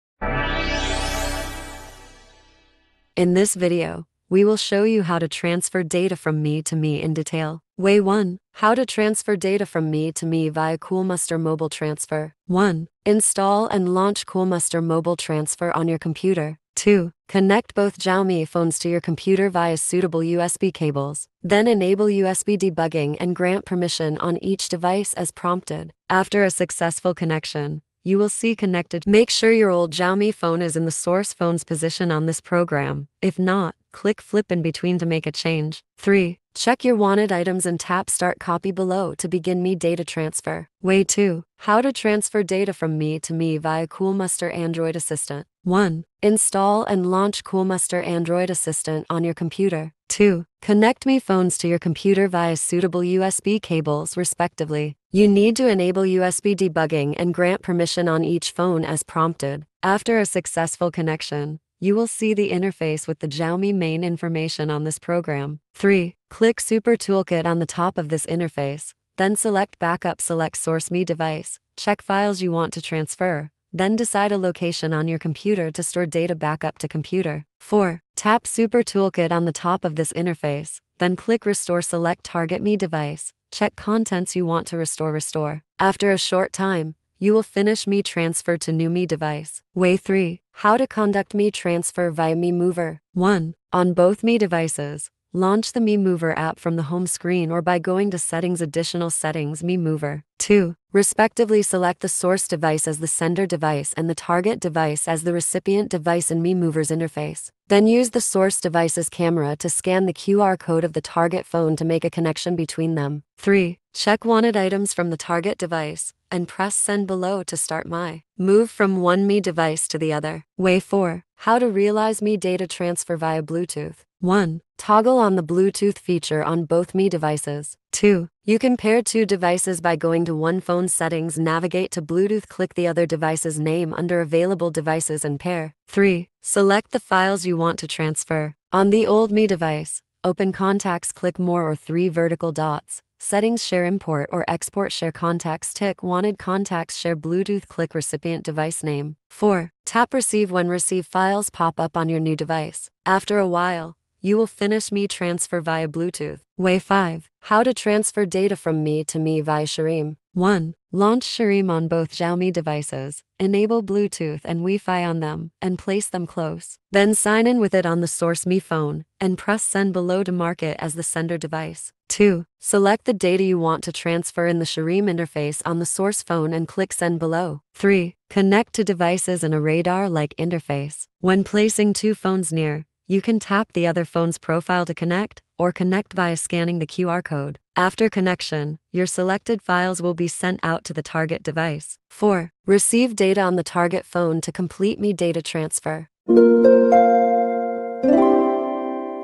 How to Transfer Data from Mi to Mi [5 Proven Methods]
Eager to conduct Mi transfer but don't know how to transfer data from Mi to Mi? This detailed guidance on Mi data transfer gives you 5 practical and proven approaches. Just follow the step-by-step instructions to realize Mi move.

00:00 Start
00:07 Way 1. How to Transfer Data from Mi to Mi via Coolmuster Mobile Transfer
00:40 Way 2. How to Transfer Data from Mi to Mi via Coolmuster Android Assistant
01:29 Way 3. How to Conduct Mi Transfer via Mi Mover
02:00 Way 4. How to Realize Mi Data Transfer via Bluetooth
02:36 Way 5. How to Transfer Data from Mi to Mi via ShareMe